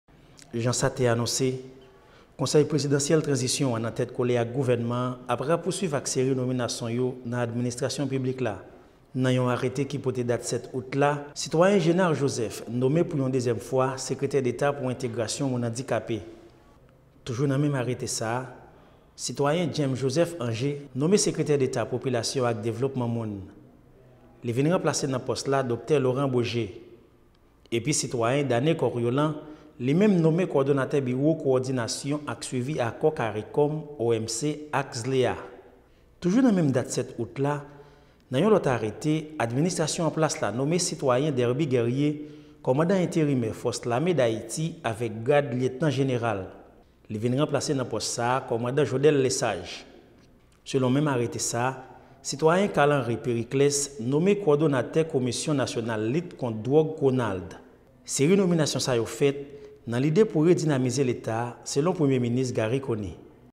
CPT/ Primati - Piblikasyon yon arete ki nome plizyè pèsonalite nan administrasyon piblik la
Pouvwa an plas la ap rapouswiv ak seri nominasyon yo nan administrasyon piblik la.

Aprè yon Konsèy minis ki fèt mèkredi 7 out lan, gouvènman an, nan tèt kole ak Konsèy prezidansyèl tranzisyon an, nome epi fè pibliye nan jounal ofisyèl Le Moniteur non divès pèsonalite ki pral pran tèt yon seri enstitisyon leta.